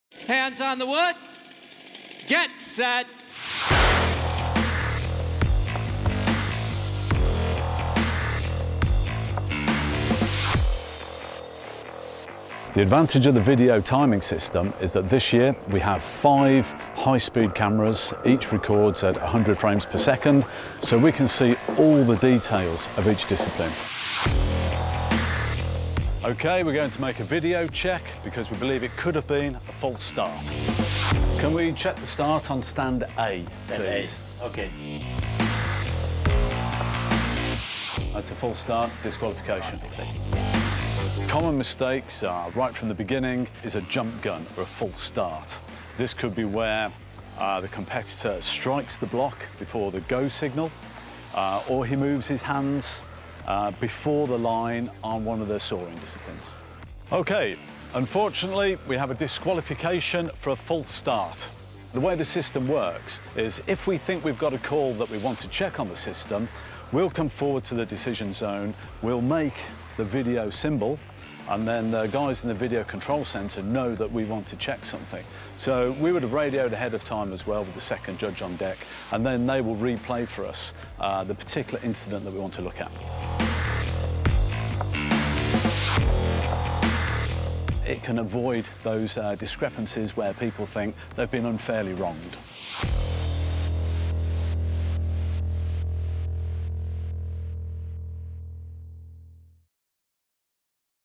Video Timing System // World Championship 2017
5 high-speed cameras recording at a 100 frames a second means our new video timing system provides split second accuracy. Find out more in this video!